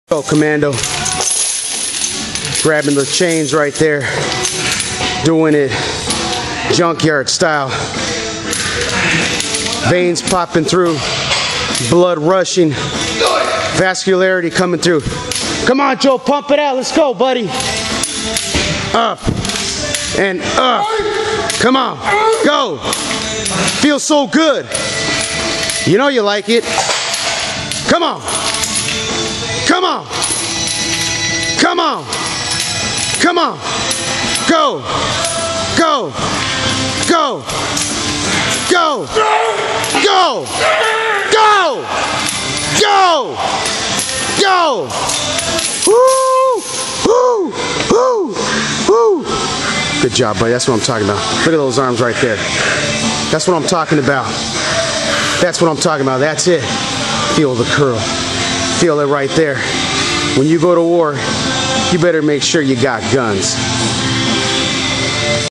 Chain curls!!!
Commando tearing it up and out of control! This is how we like it!